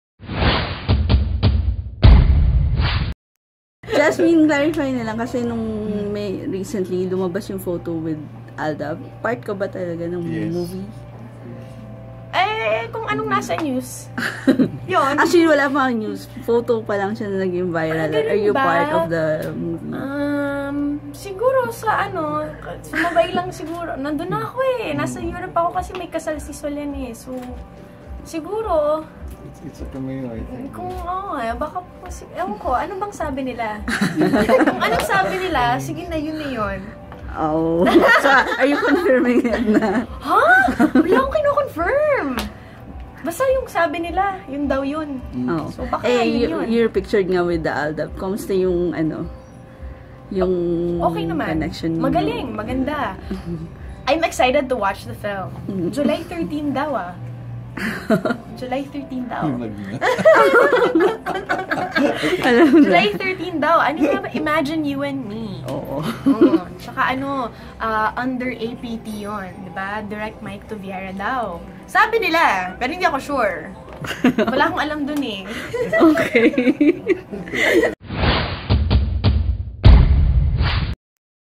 Jasmine Curtis Smith excited to watch Imagine You and Me
Jasmine Curtis Smith neither confirms nor denies participation in AlDub movie